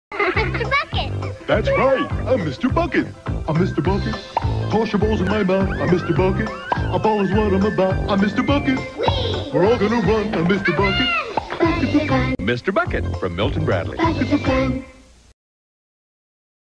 Mr Bucket Balls in My Mouth
The Mr. Bucket commercial but it's edited to make it sound like Mr. Bucket is saying 'put your balls in my mouth'.